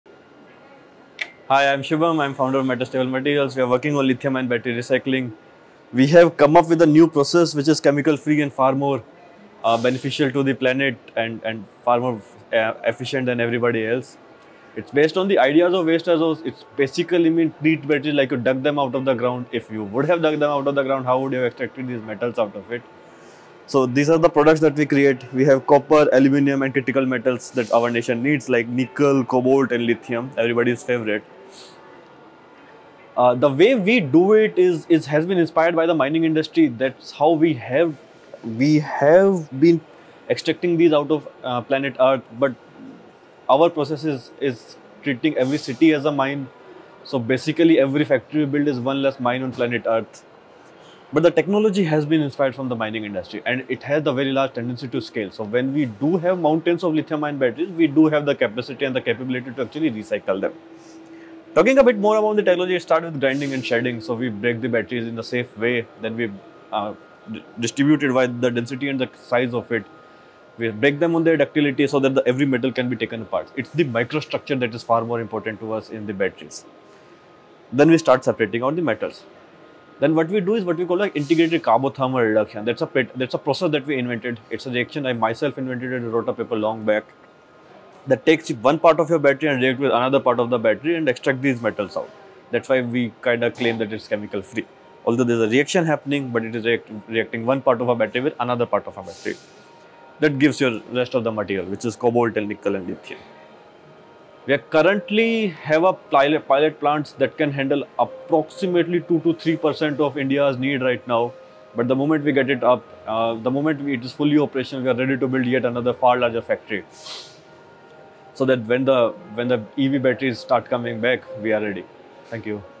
SCALE@CHEMTECH 2024 {Metastable Materials}
This is footage from the booth of Metastable Materials
They are working on Recycling Li Ion Battery without chemicals